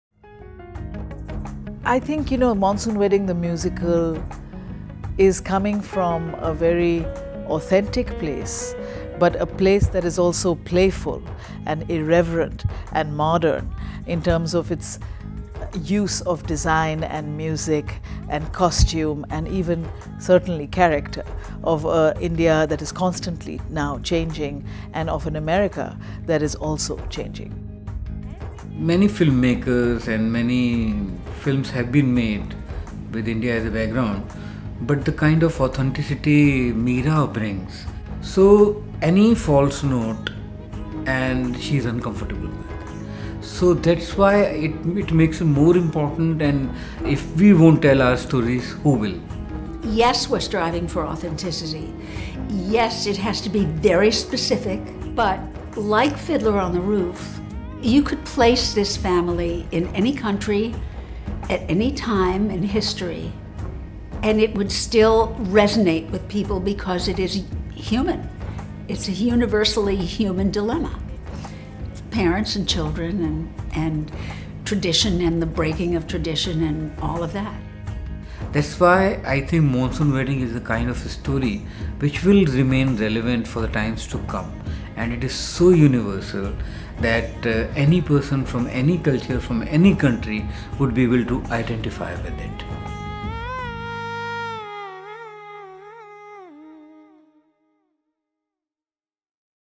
“If we won’t tell our stories, who will?”
Delve deeper into Monsoon Wedding with director Mira Nair, composer Vishal Bhardwaj, and lyricist Susan Birkenhead. Info/tickets